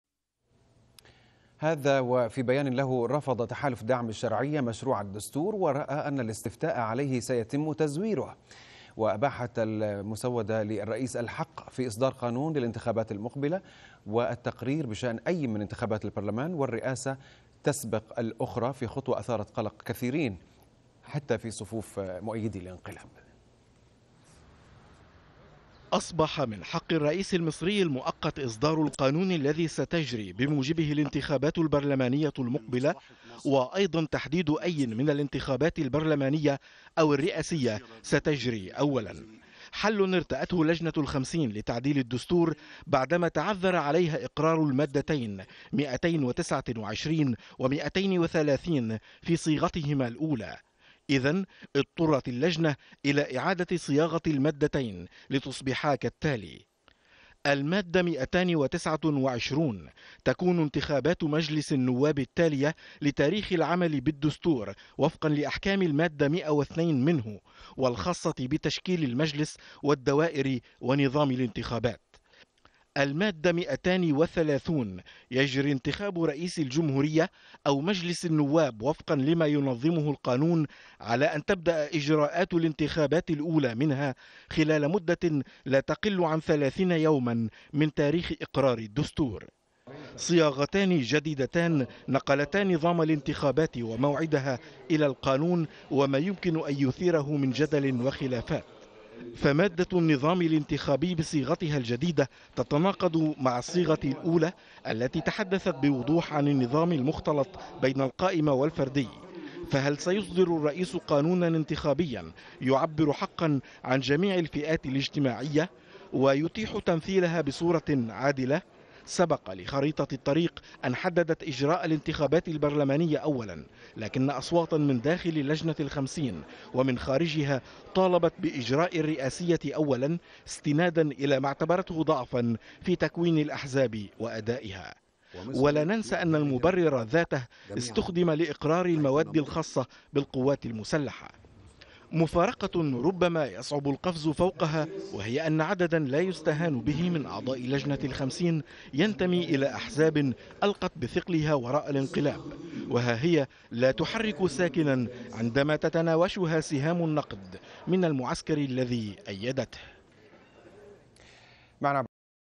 الاستفتاء على الدستور تقرير عمر الخطيب قراءة رائد بدوي
هذا وفي بيان له رفض  تحالف دعم الشرعية مشروع الدستور ورأى أن الاستفتاء عليه سيتم تزويره.
واباحت المسودة للرئيس الحق في اصدار قانون للانتخابات المقبلة والتقرير بشأن اي من انتخابات البرلمان والرئاسة تسبق الاخرى في خطوة اثارت قلق كثيرين حتى في صفوف مؤيدي الانقلاب، تقرير عمر الخطيب، قراءة رائد بدوي